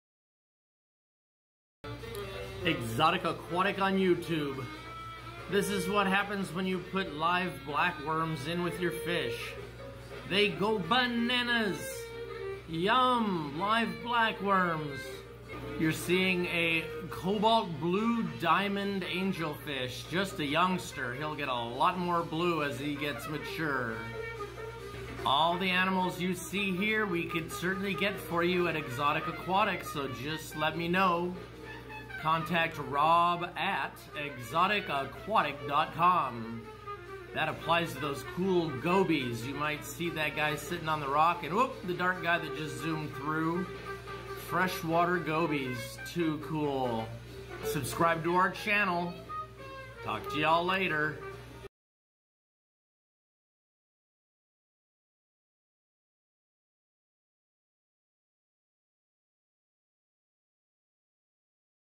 Freshwater Fish for 09/26/2020
Rob shows us how fish love live blackworms and introduces the gold nugget plecostomus, cobalt blue diamond angelfish, freshwater gobies, and many more at Exotic Aquatic.